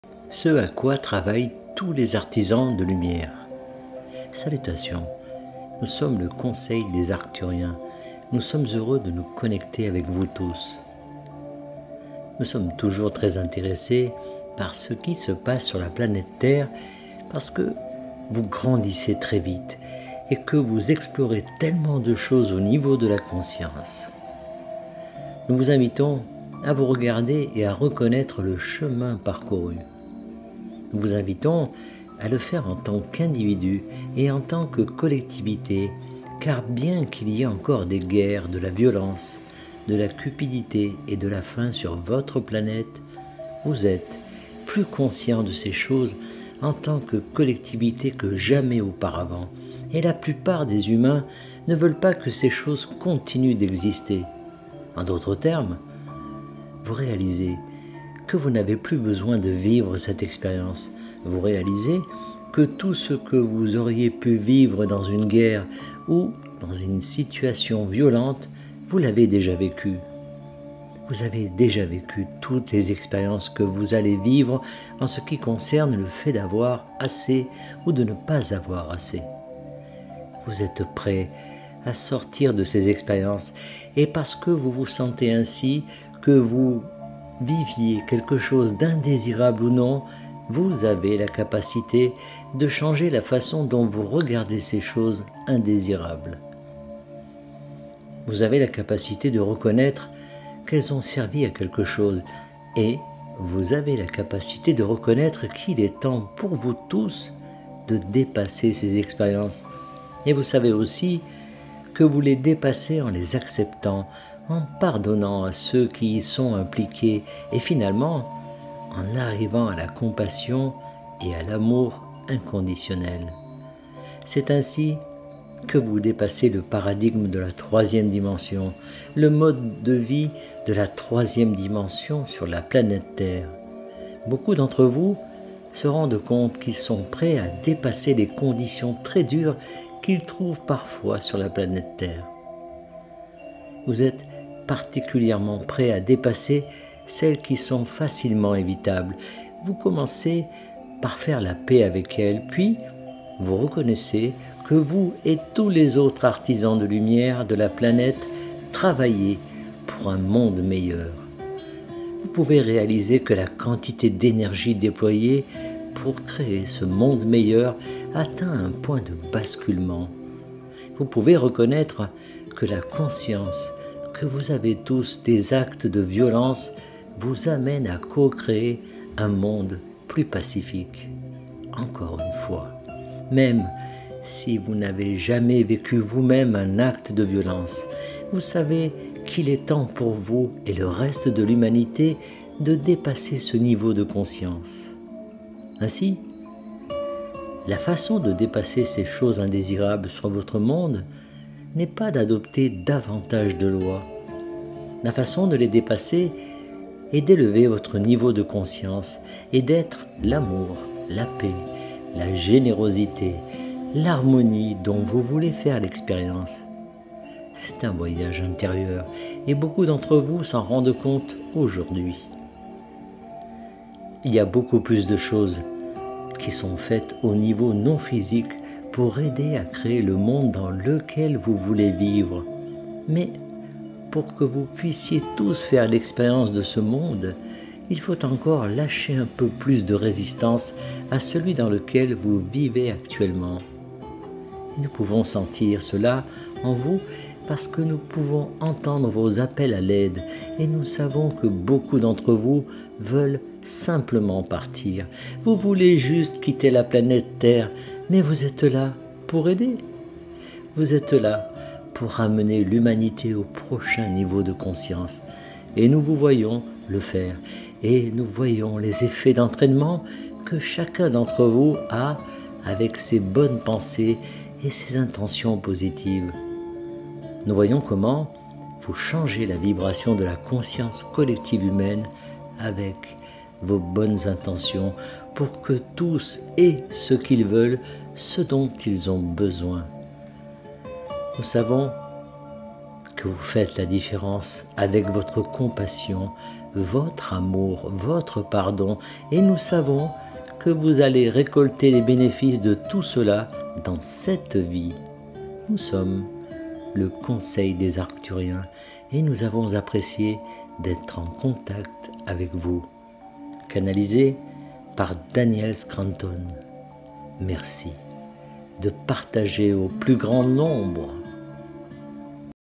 Ce à quoi travaillent tous les artisans de lumière
Salutations. Nous sommes le Conseil des Arcturiens. Nous sommes heureux de nous connecter avec vous tous.